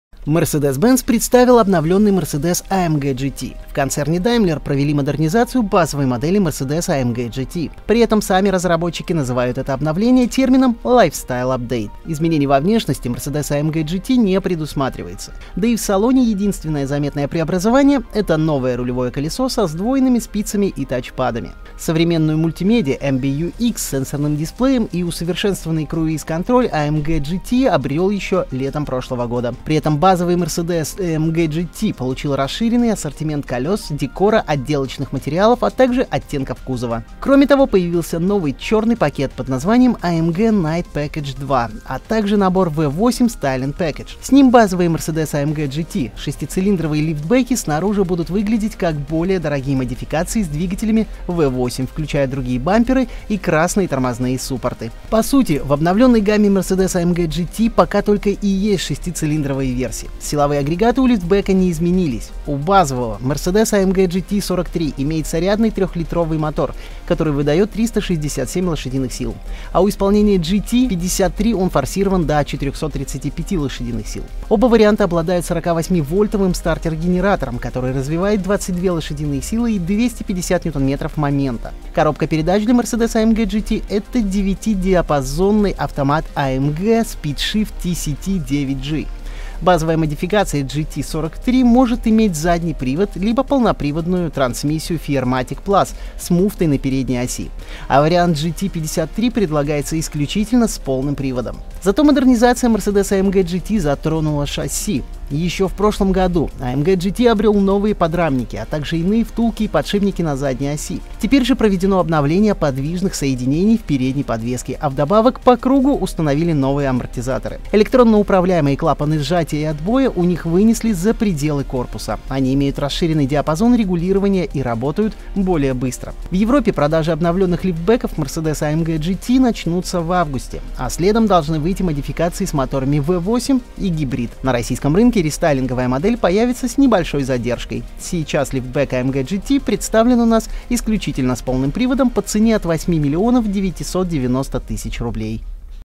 Mercedes-Benz представил обновленный Mercedes-AMG GT
На европейском рынке продажи обновленных базовых лифтбеков Mercedes-AMG GT стартуют в августе нынешнего года.